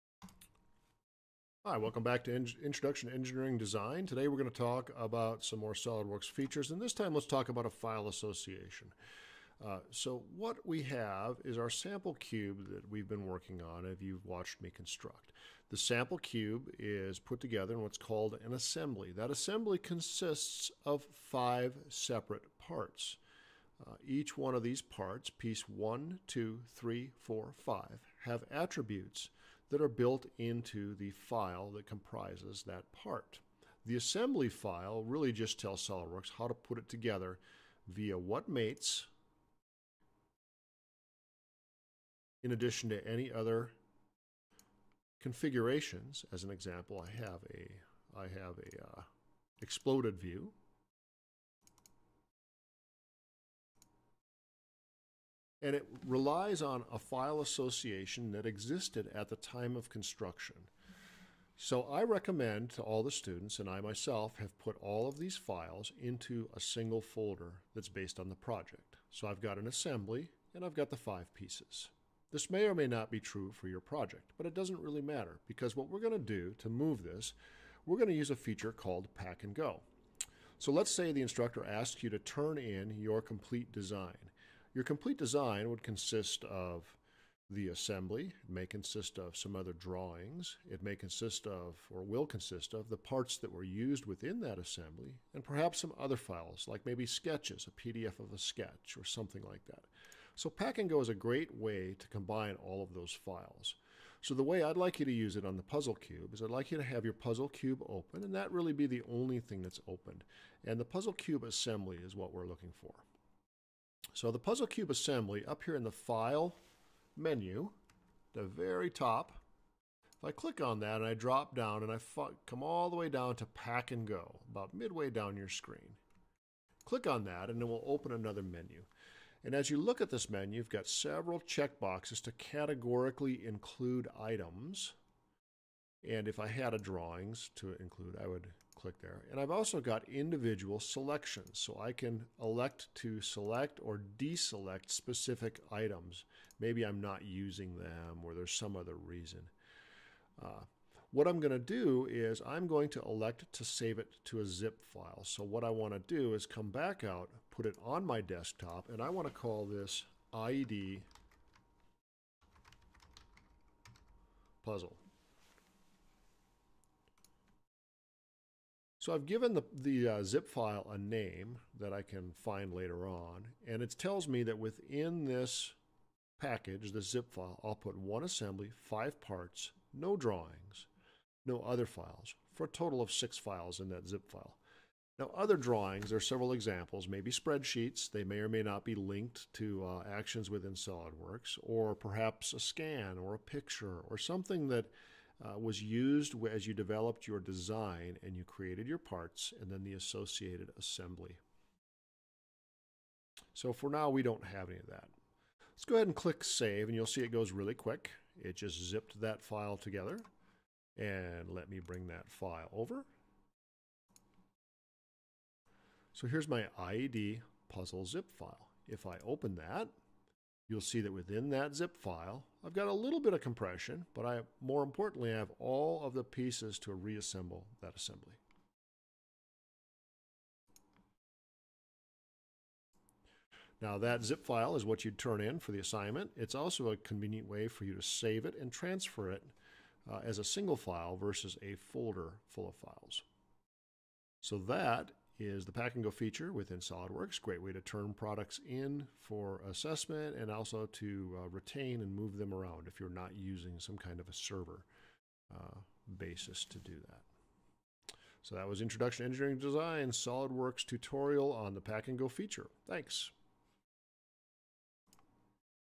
IED 4.1 Pack And Go (Solidworks)
Introduction to Engineering Design Puzzle Cube.  How to use Pack and Go features of Solidworks to move assemblies and retain associations.